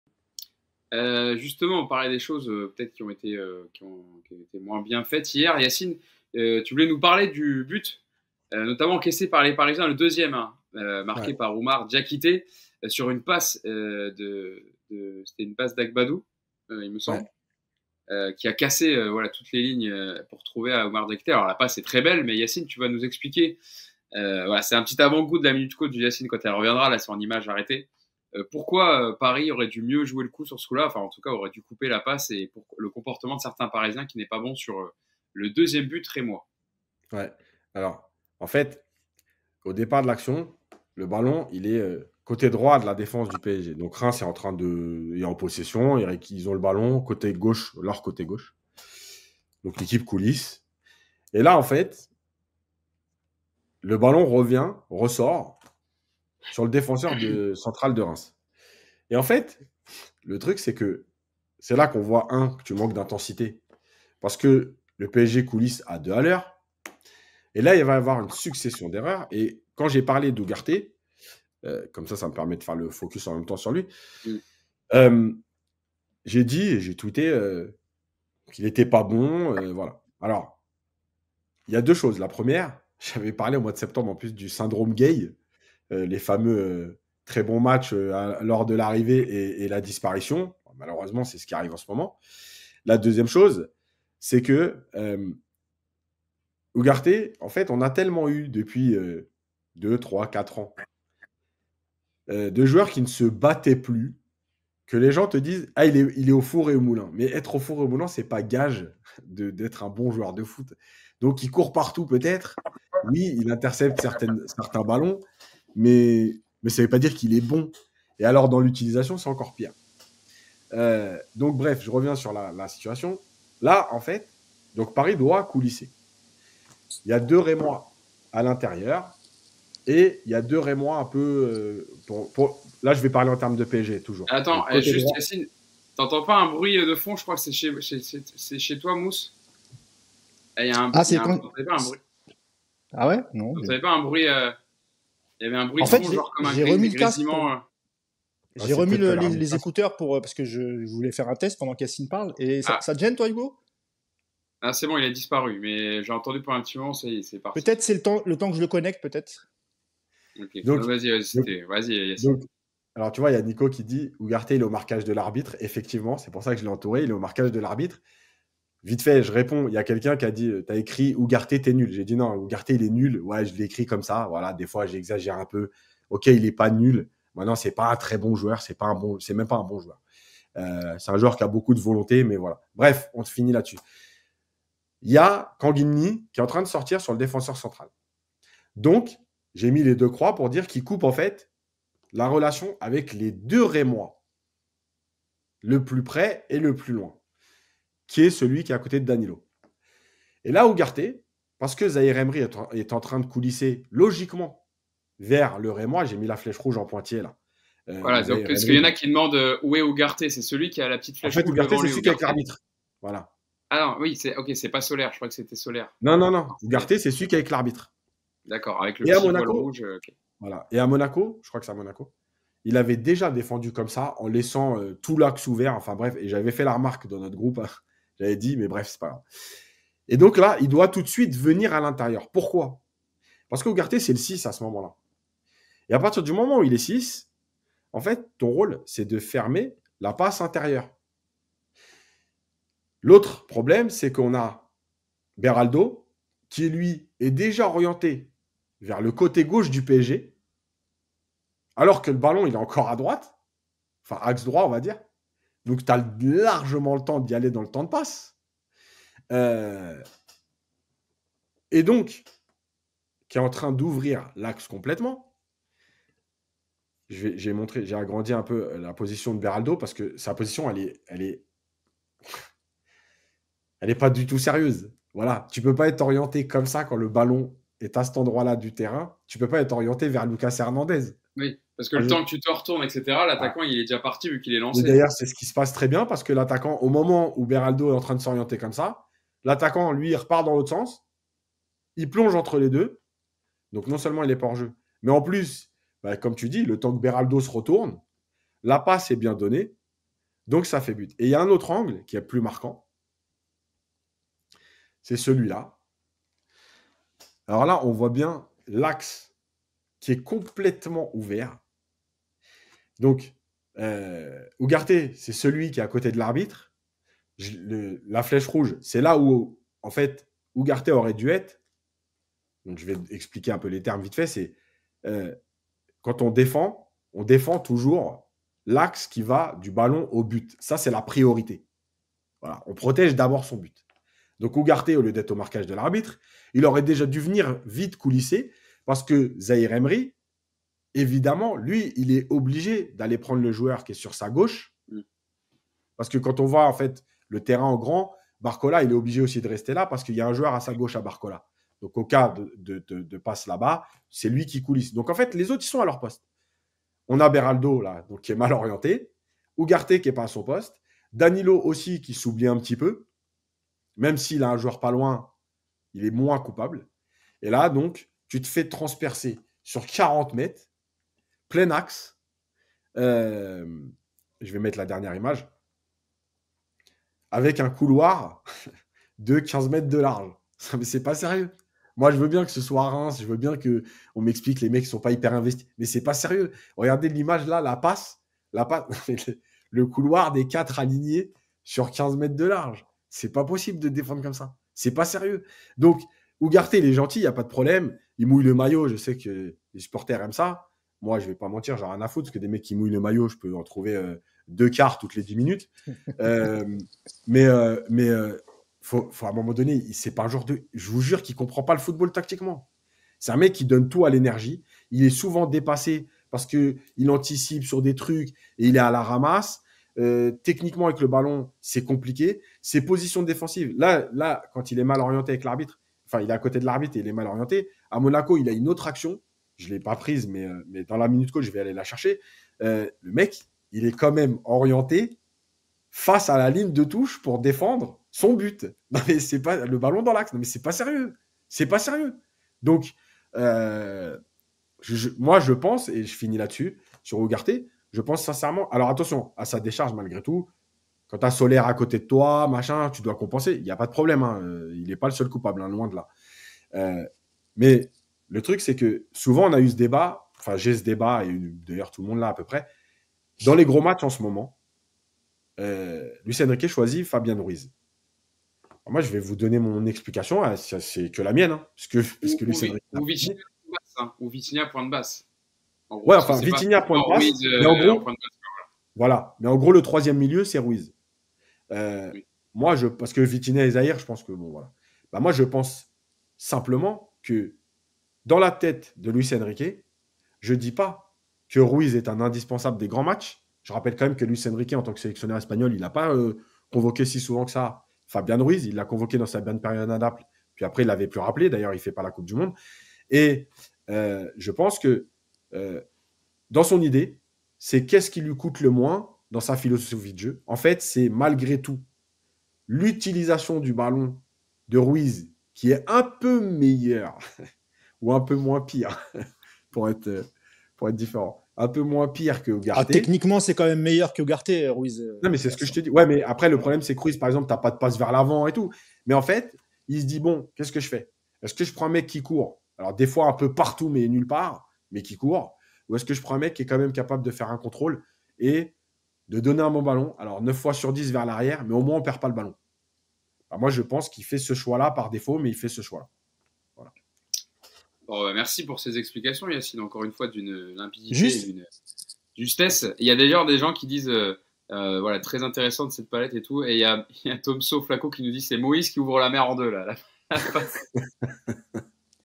PSG - REIMS - ANALYSE TACTIQUE DE YACINE - PARIS UNITED
Analyse tactique de Yacine sur le deuxième but rémois